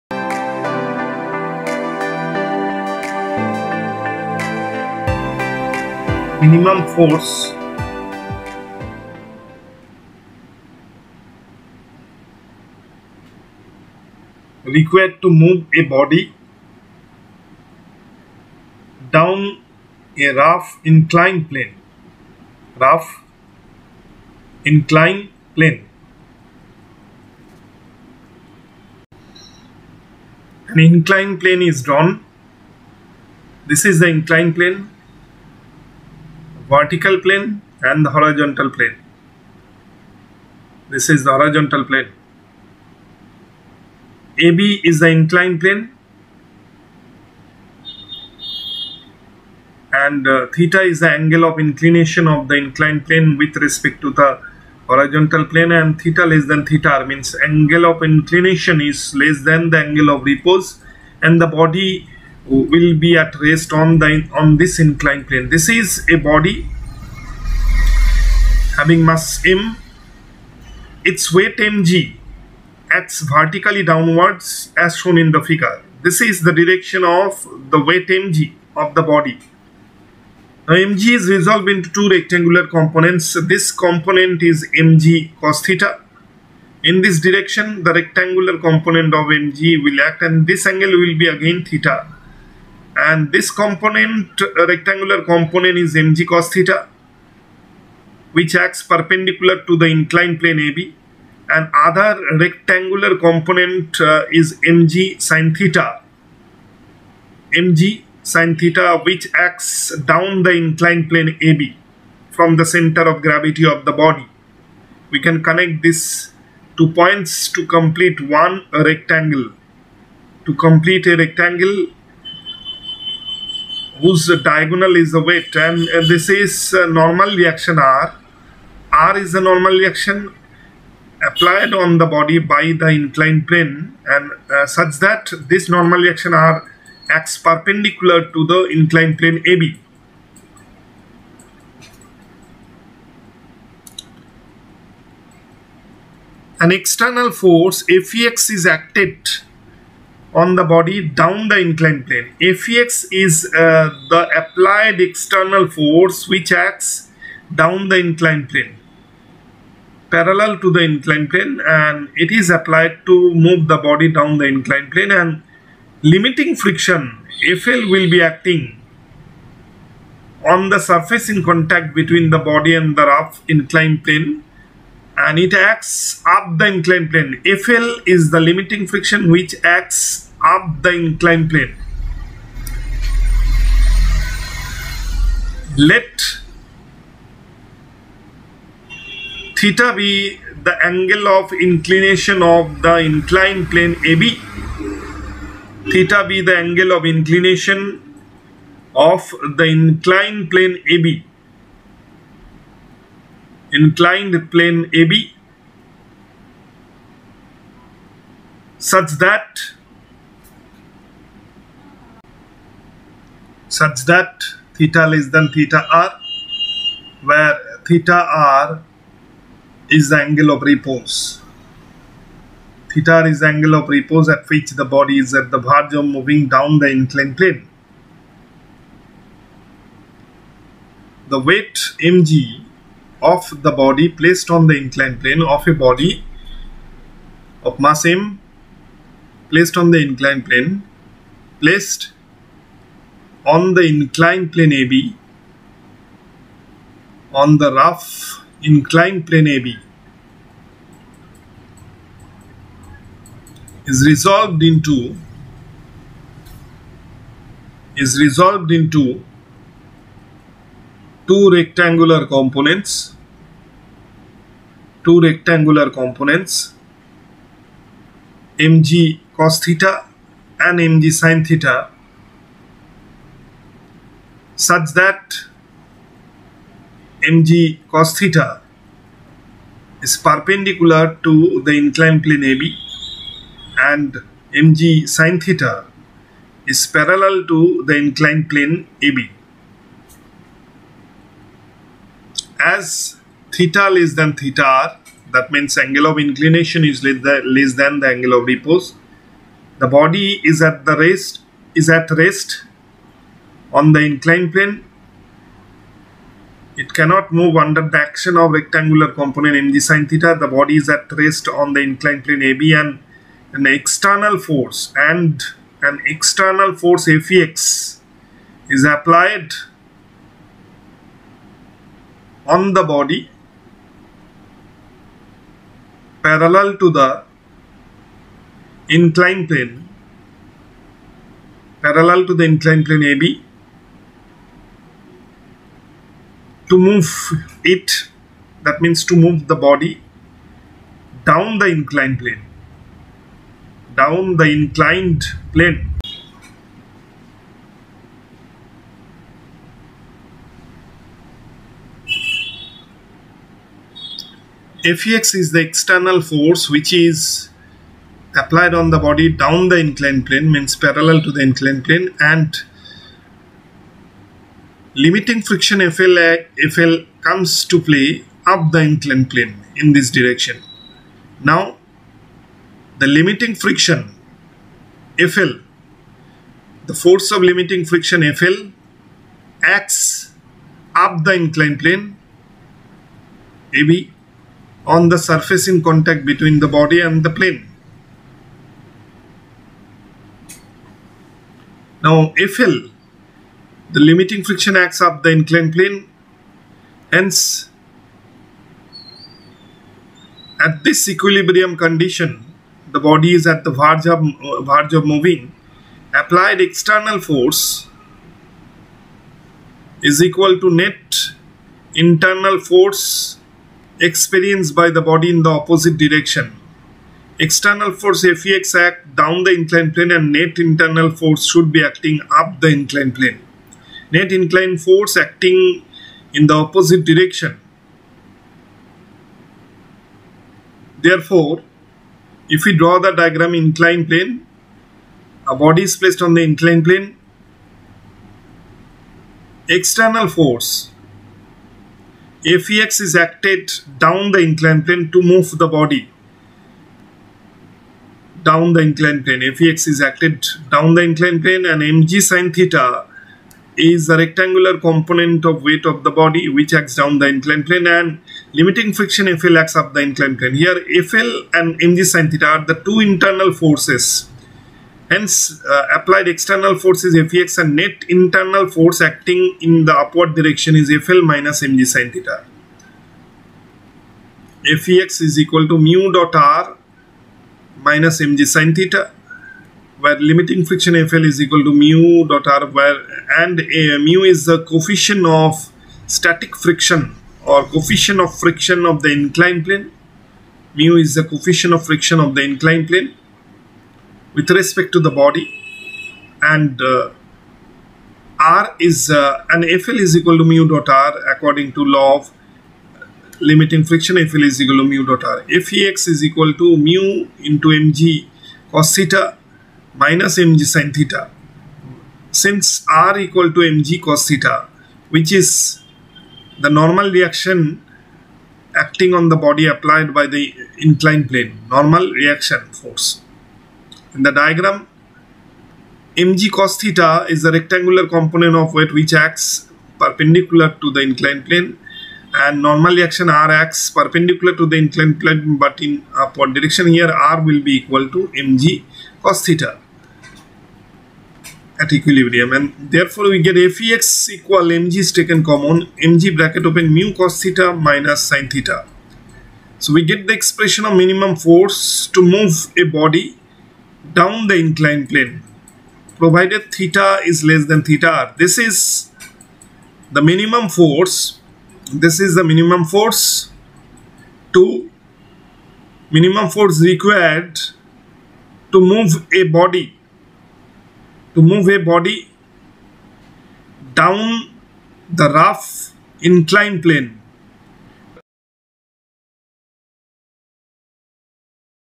Minimum fore required to move a body down a rough inclined plane
Chapter: Friction :: Find the expression of minimum force to move a body down a rough inclined plane :: If the angle of inclination (θ) is less than the angle of repose(θr), a body is at rest on the inclined plane. When the body is at the verge of moving down the inclined plane due to application of an external force Fex ,
Applied external force = Net internal force in the opposite direction ;
Fex= F  - mgsinθ ; Fex = mg( μ cosθ - sinθ) ; Where the limiting friction is F = μ.R which acts up  the inclined plane   & R is the normal reaction. ,